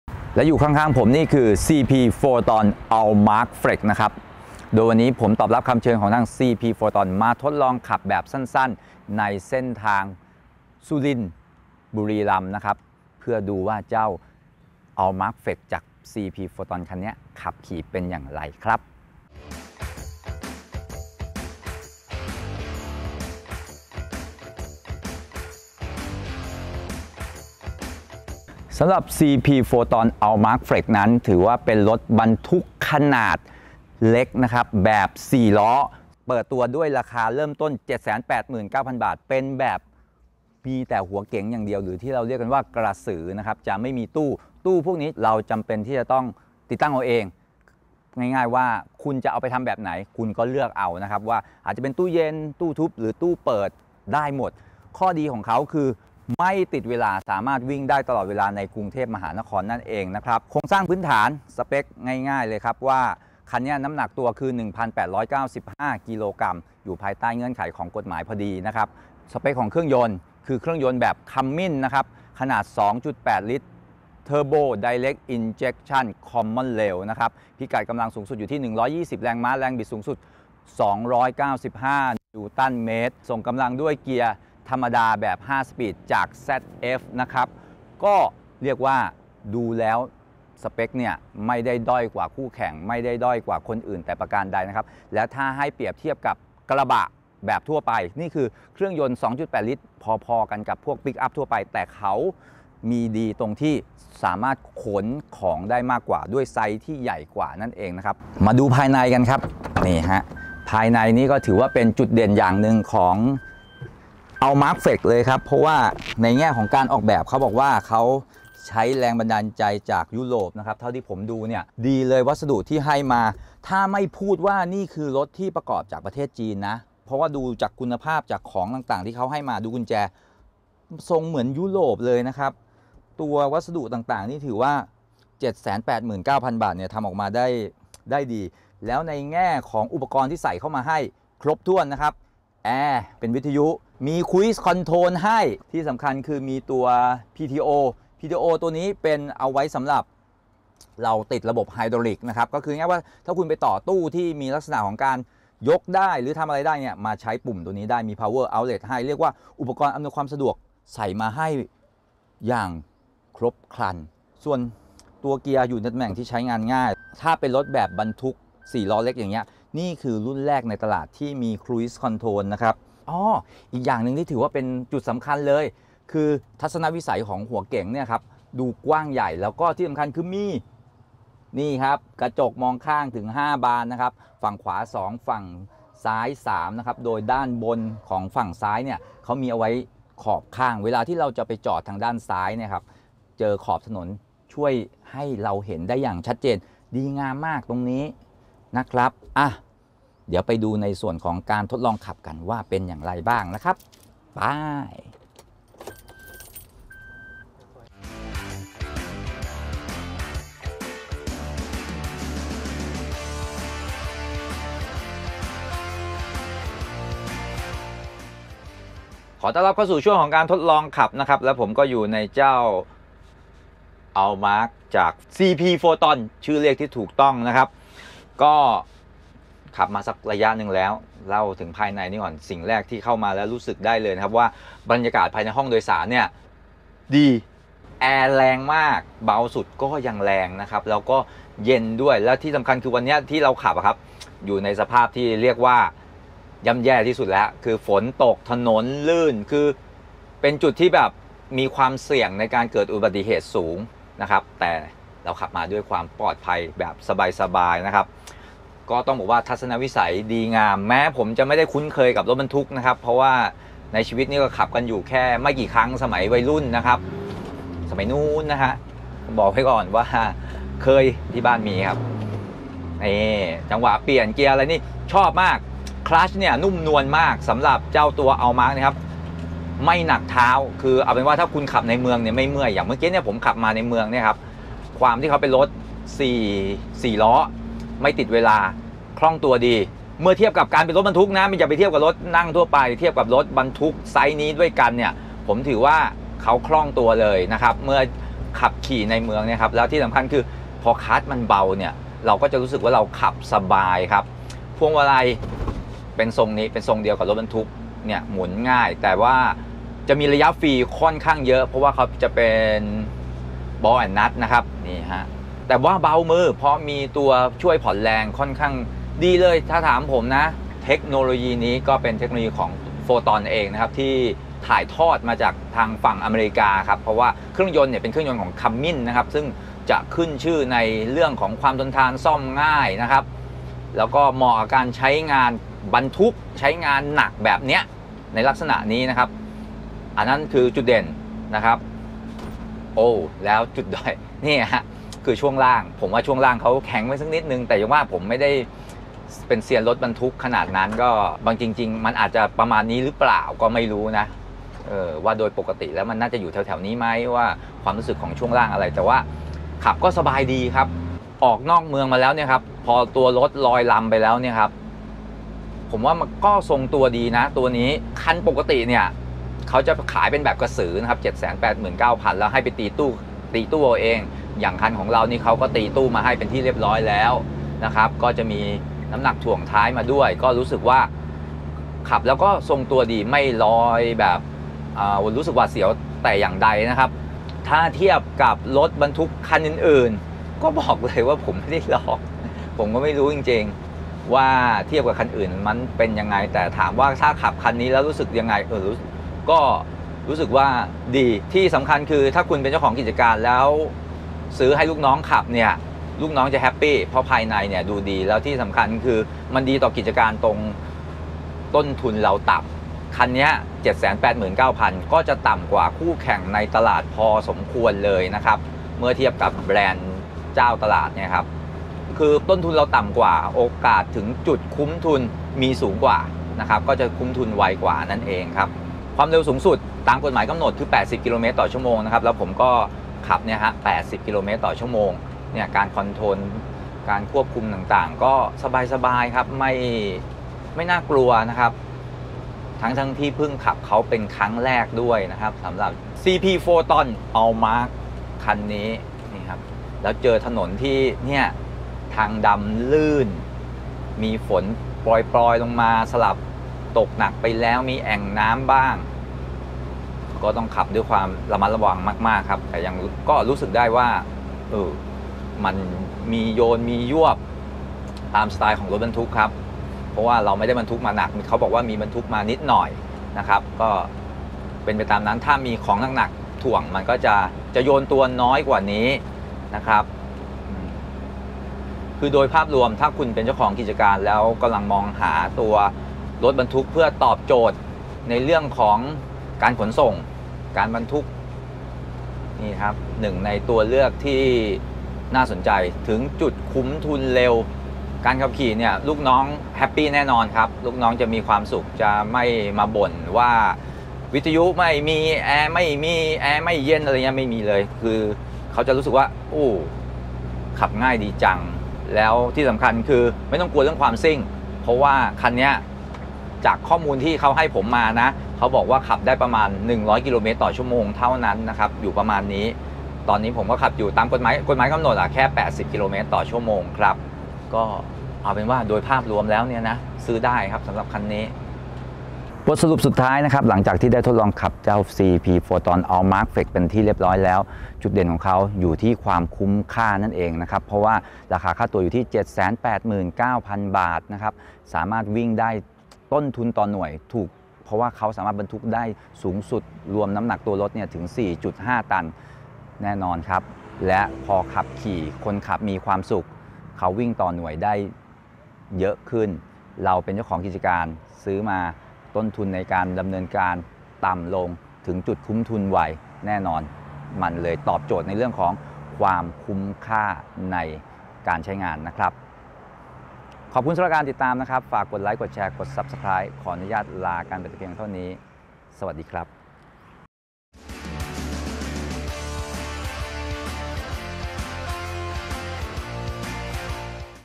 ลองขับ CP Foton Aumark Flex สี่ล้อเล็กไม่ติดเวลา : MGR Motoring
หนึ่งในตัวเลือกของผู้ประกอบการขนส่ง CP Foton Aumark Flex สี่ล้อเล็กไม่ติดเวลา เครื่องยนต์ไม่แรงเพราะ เน้นความทน วิ่งต่อรอบขนได้เยอะ ในค่าตัวที่ถึงจุดคุ้มทุนไว